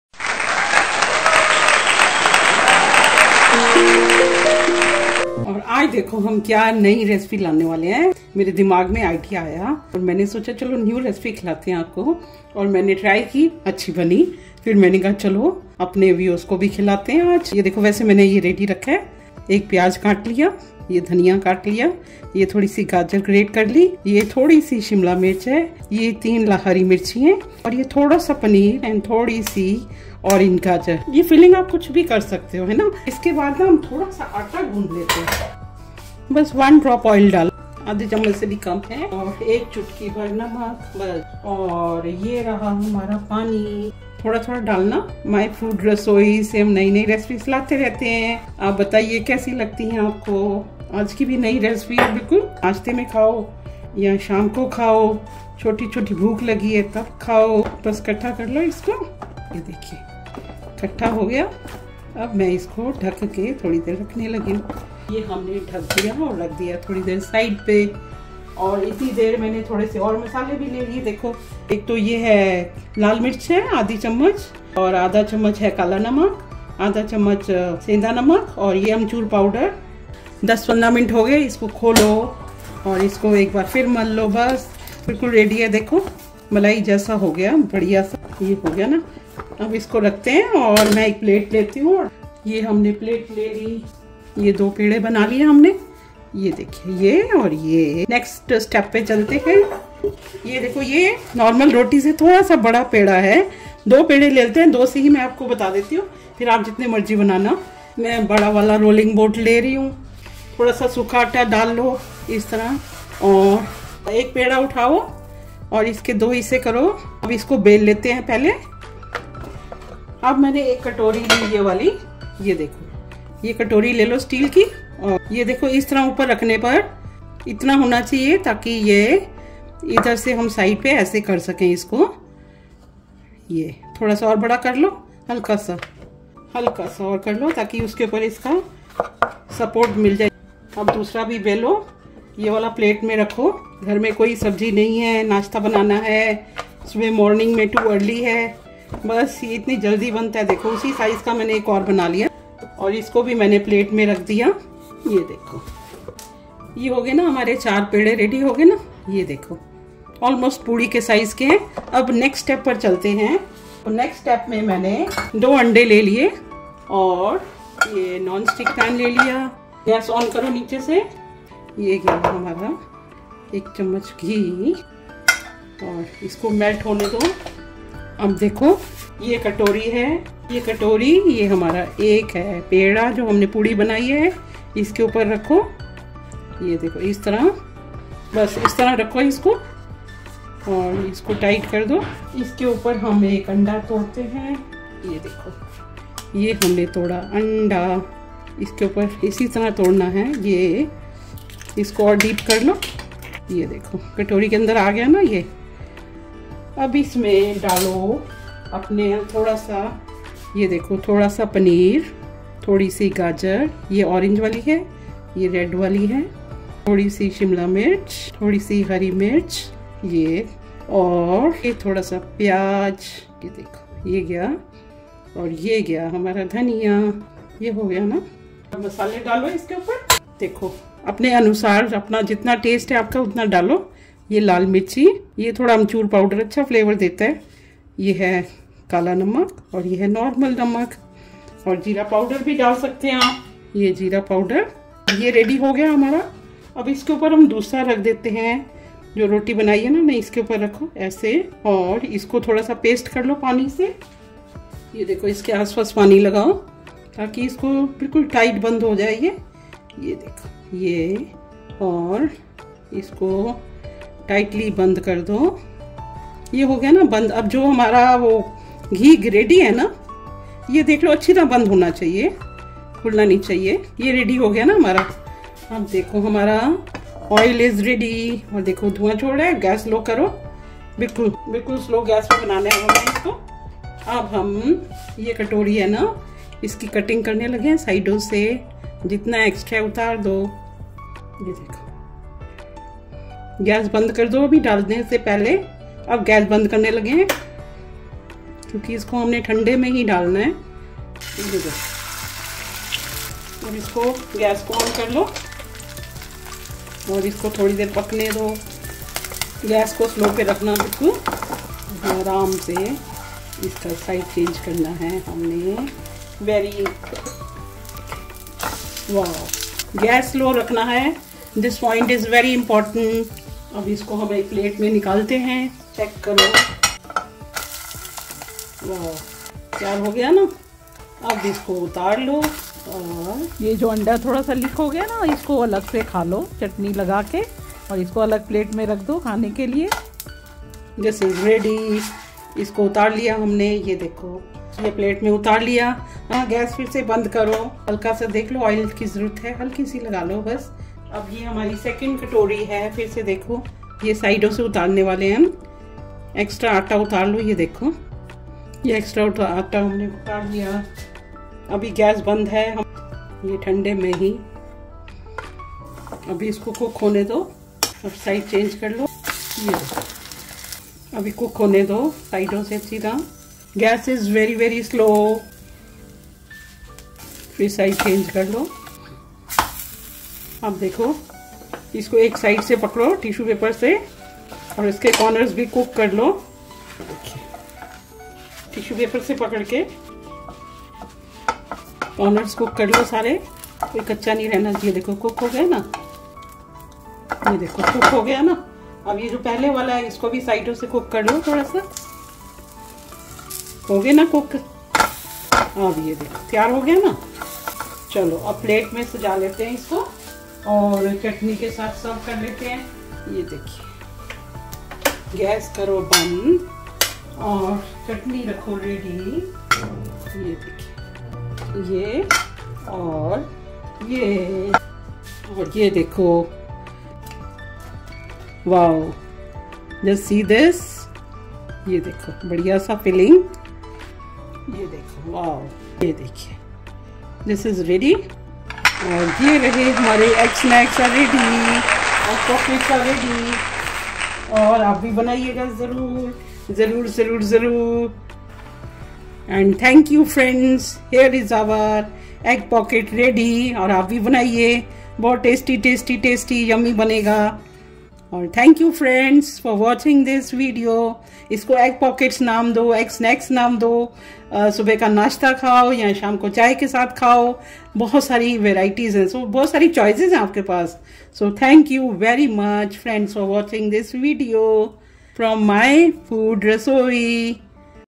Egg Pockets। Morning or Tea Time Snacks । 5 Minutes। Tiffin Snacks। अण्डा परांठा। My Food Rasoi । @
How to make Egg pockets?
How to make Egg pouches?
How to make Egg parantha ?
Egg pocket recipe.
Egg Parantha Recipe.

Ingredients
Eggs...2
Wheat flour dough...4 balls
Chopped capsicum..1/2,small bowl
Onion chopped...1 small
Green chilli chopped..2
Cheese...2tsp
Paneer...1 small cube
Fresh Corainder..1/2 small bowl
Amchur powder...1/2tsp
Black salt...1/2tsp